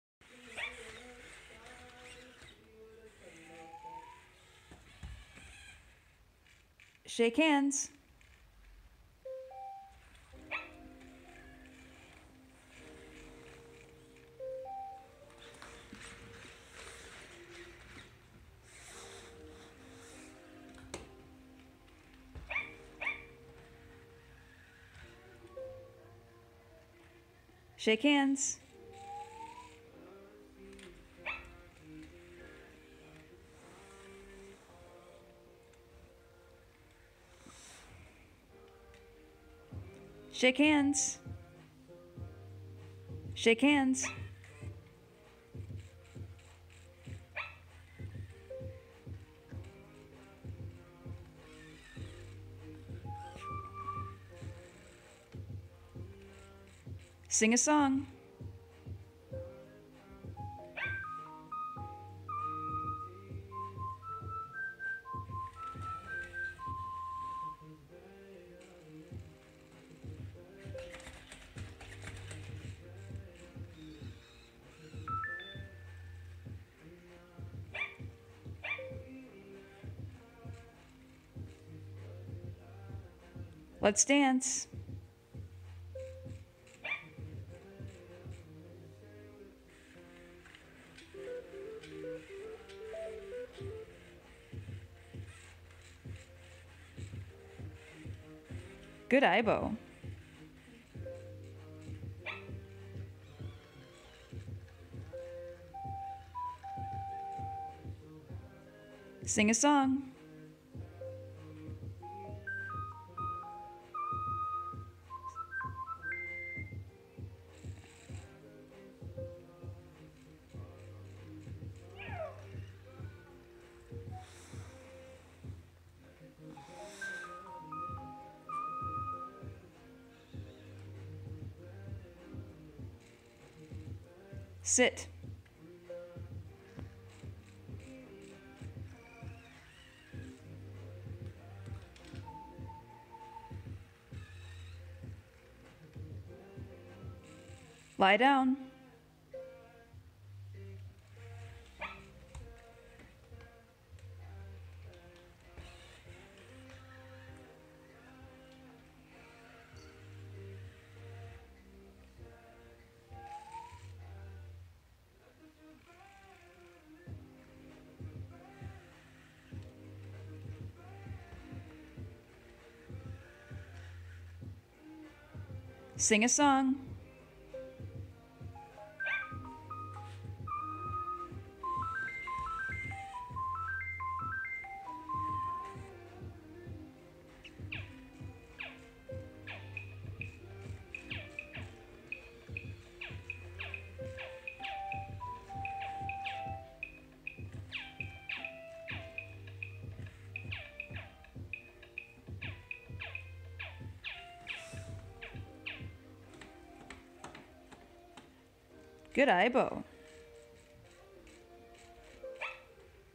Sony Aibo ERS 1000 meets Winnie the macaron.
Testing Winnie before she goes to her new home. Meanwhile Phazer is running in the background and being obstinate